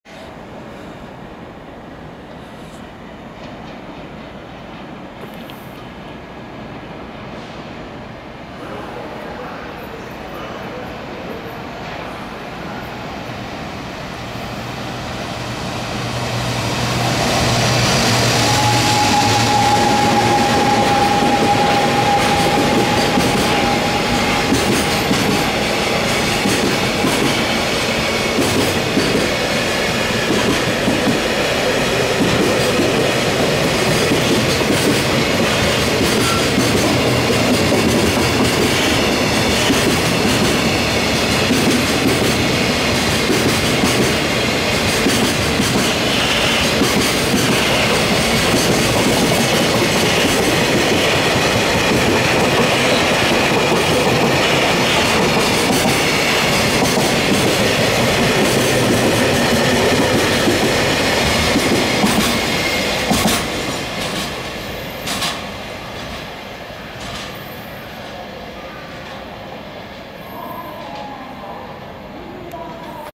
EF210形貨物列車 小田原駅通過
EF210形電気機関車(愛称：桃太郎)が牽引するコンテナ貨物列車が小田原駅を通過するシーンです。(2020年1月撮影)